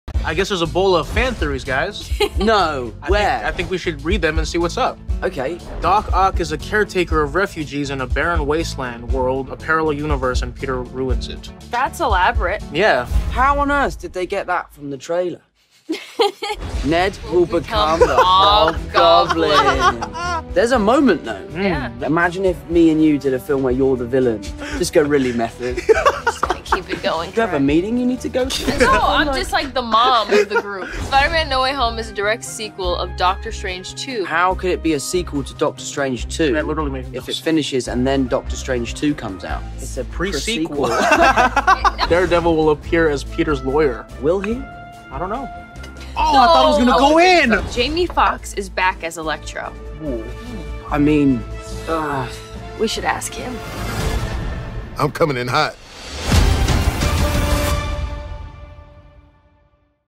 SPIDER-MAN: NO WAY HOME || FAN THEORIES || KHỞI CHIẾU 17.12.2021
Tóm tắt clip: "Giả thuyết vẫn chỉ là giả thuyết" 🤣 Chờ ngày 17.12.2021 ra rạp để biết kết quả mọi người nhé.